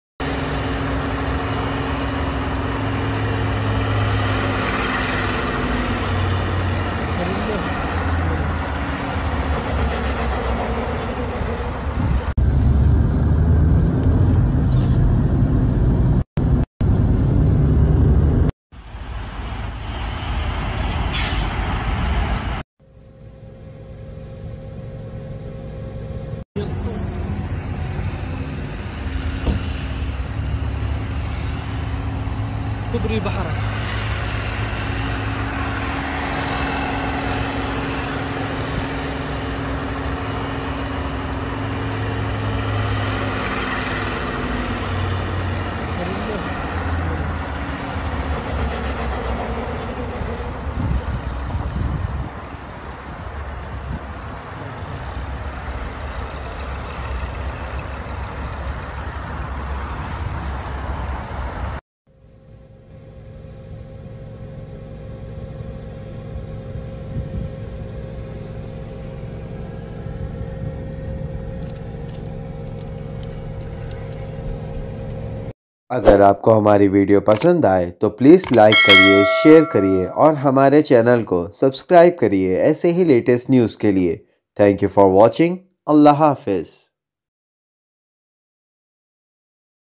A Terrible Accident Of Two Trucks On Makkah Road On Bridge kubri
Arabia latest news in Evening with hasan Rayeen | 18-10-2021 | Saudi news Today

Dosto is video me maine aapko Saudi Arabia ki latest news dene ki koshish ki hai umeed hai
appko ye video pasand ayegi.